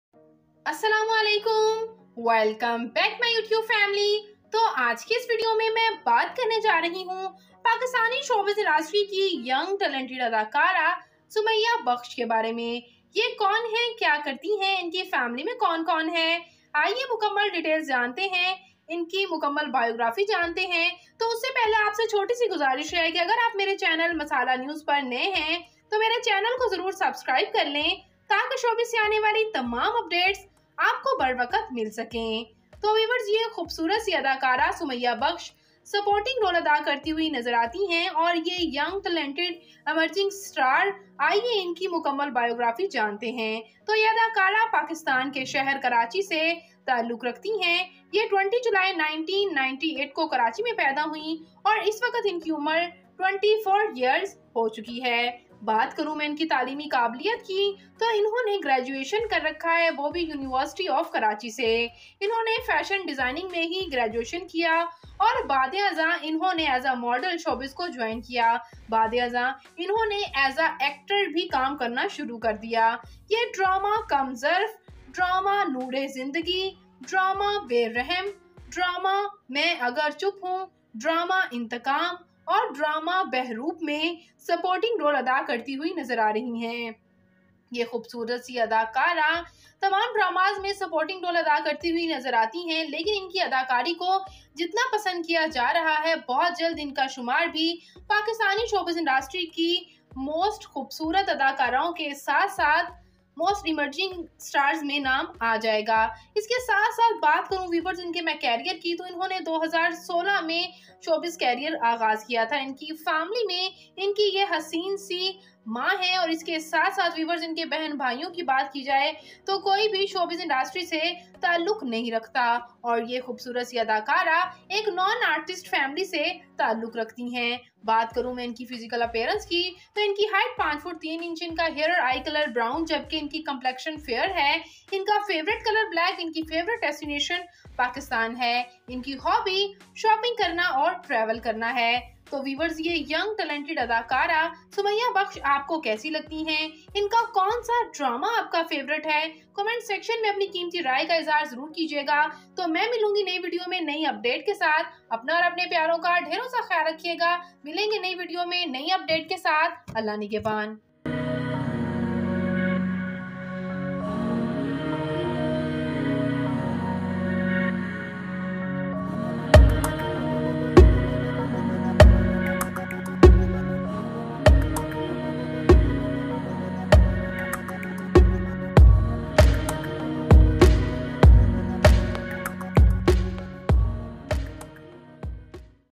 Sumaiya Bukhas Husband Mother Father Daughter Family Biography - Masala News
Sumaiyya Bukhsh
Born 20 July 1998 (age 24)
Karachi, Pakistan
Education University of Karachi
Occupation(s) Actress, Model
Sumaiyya Bukhsh is Pakistani actress and model.[1] She is known for her roles in dramas such as Kam Zarf, Main Agar Chup Hoon, Inteqam, Noor-e-Zindagi and Be Rehem.[2][3]

Early life
Sumaiyya was born on July 20, 1998 in Karachi, Pakistan and she completed her studies from University of Karachi.

Career
She made her debut as an actress in 2016 on Geo TV.[4] She did a supporting role in drama Noor-e-Zindagi as Mano and Rishtay Kachay Dhagoon Se as Hareem .[5] In 2017 she did a lead role in drama Jalti Barish as Sarwat an innocent girl, she was praised for her natural acting and expressions.[6] In 2018 she did three lead roles in dramas, she played as Hina with Rizwan Ali Jaffri in drama Mera Ghar Aur Ghardari who gets mistreated by her husband's first wife.[7] She played lead role along with Omer Shahzad in Be Rehem drama as Ashi, who is abused and tortured by her husband and her pairing with Omer Shahzad became popular.[8] She also played lead role in drama Rabbaway as Zeerat an innocent girl who wishes to marry her lover.[9] She also did modeling for designers, magazines, commercials and advertisements.[10][11] In 2019 she played lead role as Hania in Mera Kiya Qasoor as a victim daughter who is mistreated by her father, in the same year she appeared in drama Kam Zarf as Mona the youngest sibling who wants to see her elder sister happy, she also played lead role as Maha in drama Soya Mera Naseeb.[12][13]

Filmography
Television
Year Title Role Network
2016 Noor-e-Zindagi Mano Geo TV
2016 Faltu Larki Muneeza A-Plus
2017 Rishtay Kachay Dhagoon Se Hareem A-Plus
2017 Tumhare Hain Anum ARY Digital
2017 Jalti Barish Sarwat TV One[14]
2018 Mera Ghar Aur Ghardari Hina Geo TV[15]
2018 Be Rehem Ashi Geo Entertainment[16]
2018 Rabbaway Zeerat Bol Entertainment
2019 Mera Kiya Qasoor Hania A-Plus[17]
2019 Kam Zarf Mona Geo TV[18]
2019 Soya Mera Naseeb Maha Hum TV[19]
2020 Mein Rani Fozia Express Entertainment[20]
2020 Chamak Damak Rameen Hum TV
2020 Main Agar Chup Hoon Shifa Geo Entertainment[21]
2021 Jannat Chordi Meinay Mariam SAB TV
2021 Oye Motti Jugnu Express Entertainment
2021 Iman Aur Yaqeen Ayesha Aaj Entertainment
2021 Shehnai Shama ARY Digital
2021 Dikhawa Season 2 Wareesha Geo Entertainment
2021 Shehnai Kaun Bajaye Ga Shafaq SAB TV
2021 Sirat-e-Mustaqeem Azir's mother ARY Digital
2021 Makafaat Season 3 Taniya Geo Entertainment
2022 Inteqam Zoya Geo TV[22]
2022 Beqadar Umeed Hum TV
2022 Sirat-e-Mustaqeem Season 2 Komal ARY Digital
2022 Aitraaf Hadia Aan TV
2023 Meri Betiyaan Asmara Aan TV
2023 Sirat-e-Mustaqeem Season 3 Saima ARY Digital
2023 Ahsaas Aiza Express Entertainment
2023 Behroop Zara Geo Entertainment
2023 Bojh Maha Geo Entertainment
Telefilm
Year Title Role
2018 Mai Cheemi Ka Faisla Shabbo
2019 Pyar Kay Lashkaray Shabnam
2022 Ghapla Chimchim
References
 "Junaid Khan returns with new show 'Kam Zarf". GulfNews. June 20, 2020.
 "HIP Reviews Kamzarf Episode 14: Ali Ansari Plays the Character of a Classic Brother". HIP. June 8, 2020.
 "7th Sky Entertainment to bring out another love story 'Main Agar Chup Hoon'". Daily Times. December 10, 2021.
 "Junaid Khan on his next, Kasa e Zaat". The International News. June 24, 2020.
 "HIP Reviews Kamzarf's Last Episode: Nadia Khan Stole the Show with